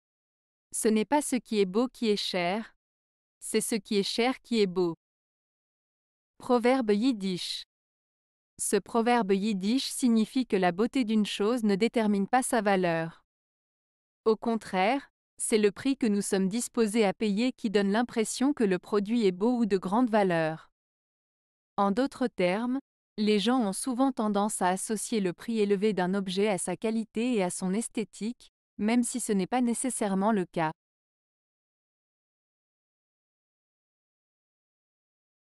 Ce n'est pas ce qui est beau qui est cher ; c'est ce qui est cher qui est beau. - Proverbe Yiddish
Ce proverbe yiddish signifie que la beauté d'une chose ne détermine pas sa valeur. Au contraire, c'est le prix que nous sommes disposés à payer qui donne l'impression que le produit est beau ou de grande valeur. En d'autres termes, les gens ont souvent tendance à associer le prix élevé d'un objet à sa qualité et à son esthétique, même si ce n'est pas nécessairement le cas.